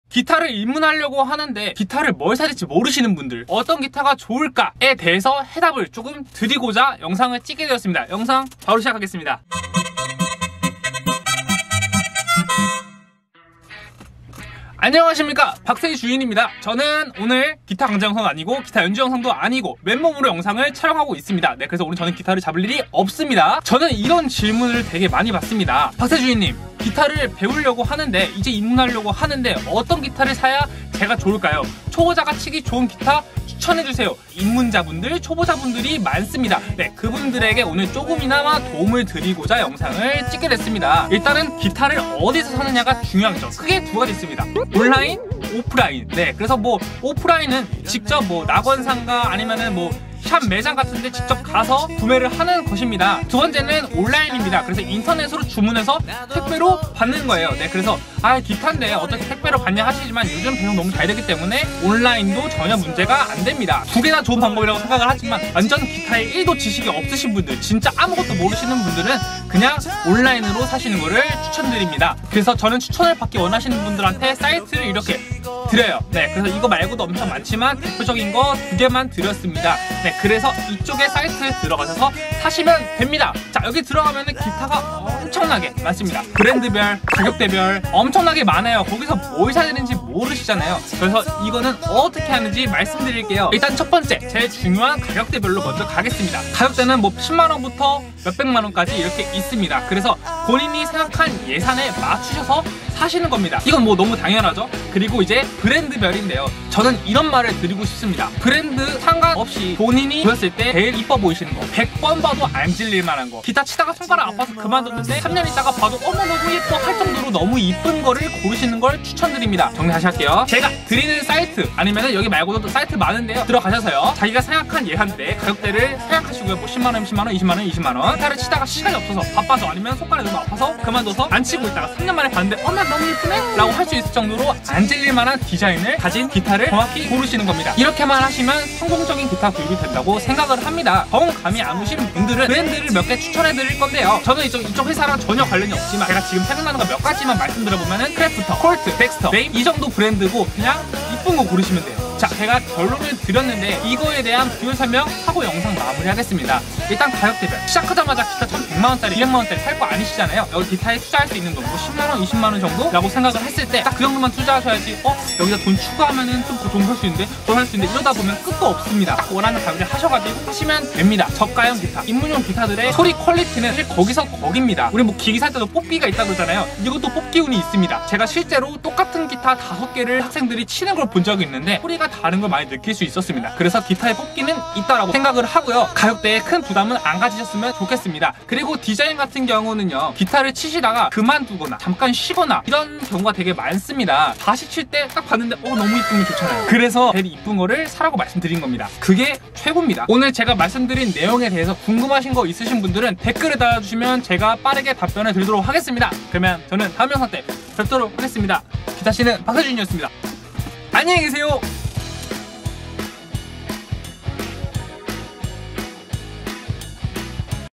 기타 사기전에 안보면 후회하는 영상
안녕하세요 ! 박새주인 입니다 :)
기타 입문하실 예정인 분들 , 기타 구매를 앞두고 계신분들
에게 도움이 조금이나마 되고자 저의 생각을 영상으로
담아 봤습니다. 생각나는 사이트 아래에 적어드리겠습니다.

(저는 위 회사들과 아~ 무 관련이 없음을 알려드립니다)